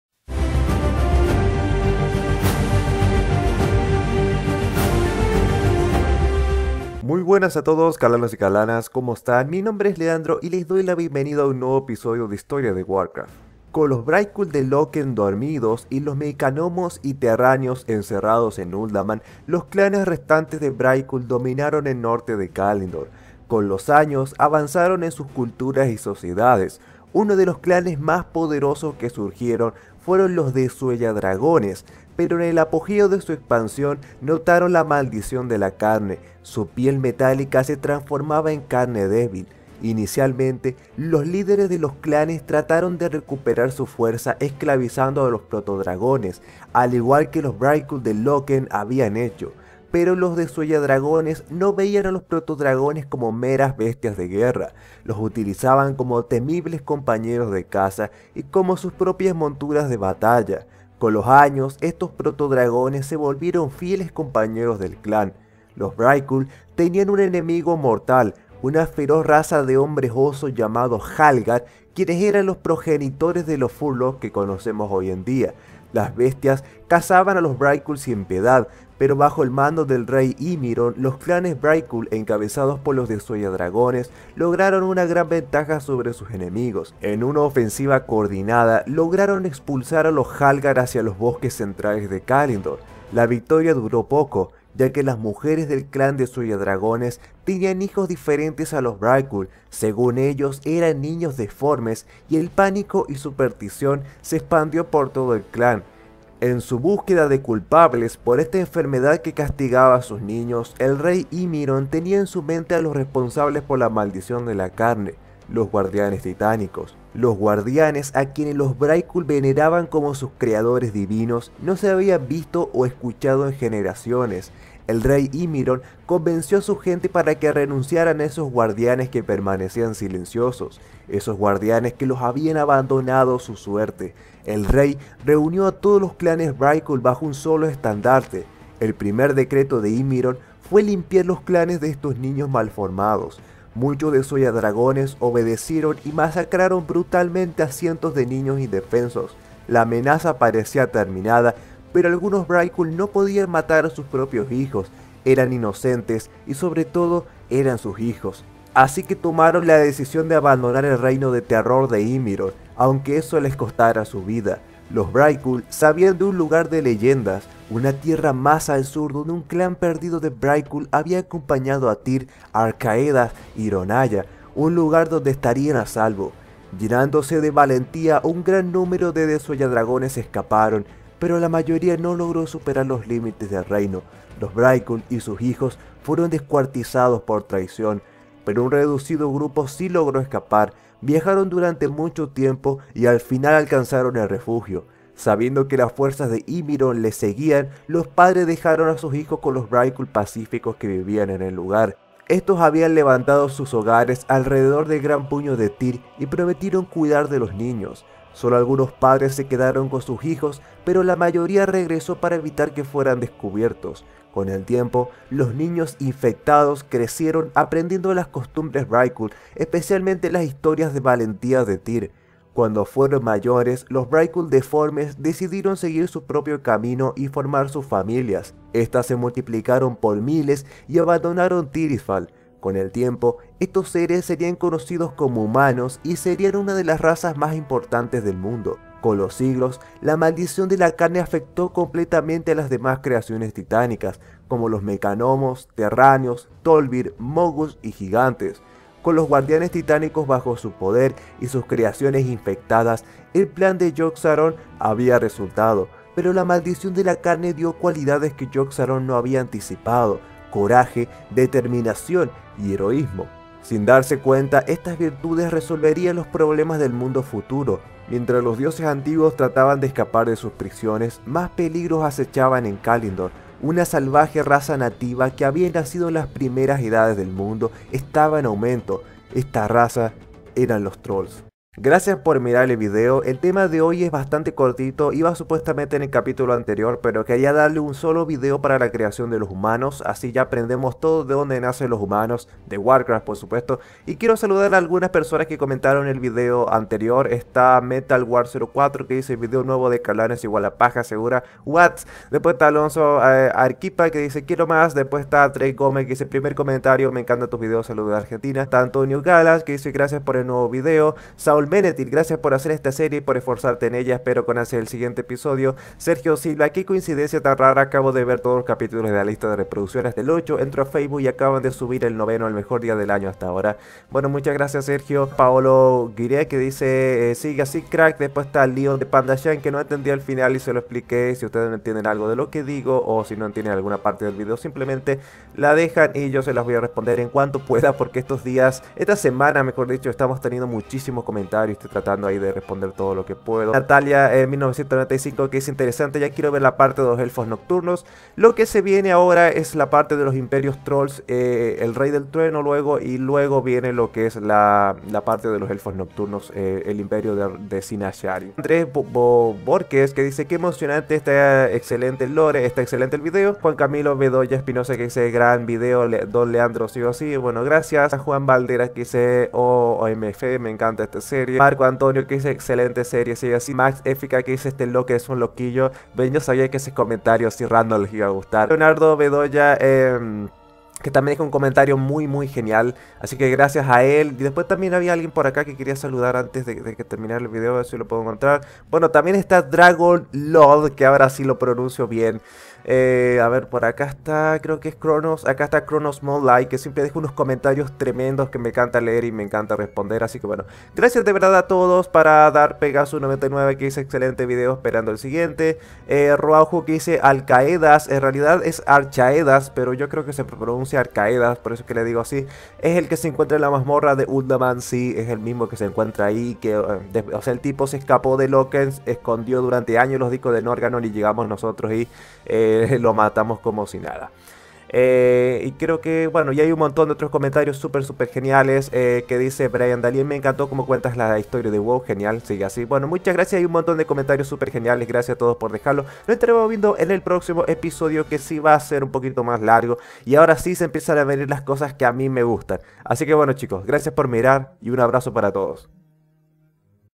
Los Clanes Vrykul/Nacimiento de los Humanos - Historia de Warcraft Crónicas #10
Mira toda la historia de Warcraft actualizada al nuevo canon

¡Gracias por mirar el vídeo! el de hoy es cortito pero es un tema que se merecía un solo vídeo para si, como lo sera la historia de cada raza del juego
Cualquier duda ya sabes donde encontrarme, aquí abajo baby 7u7